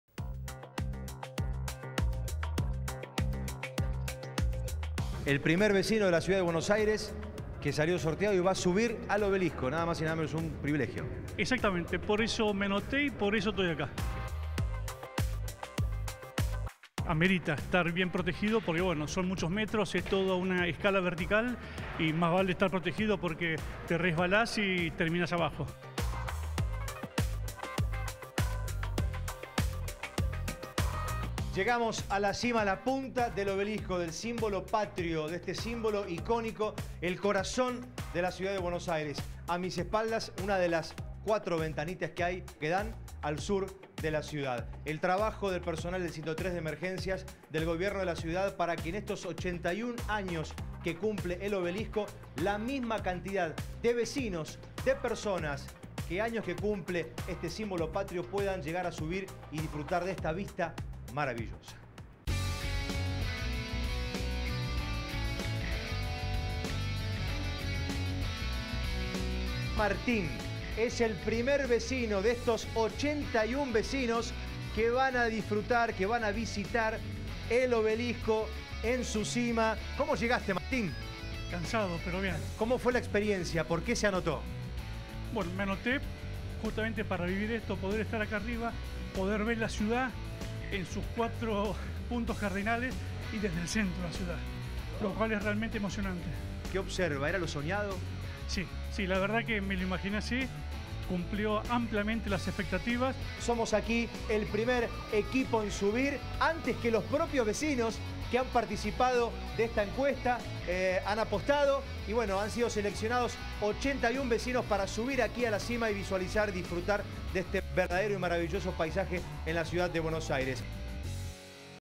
El Obelisco, de festejo - Noti20
El Obelisco, emblema de la ciudad de Buenos Aires, cumple hoy 81 años y lo festeja junto a los vecinos quienes pudieron subir a la cima y disfrutar de una vista inmejorable.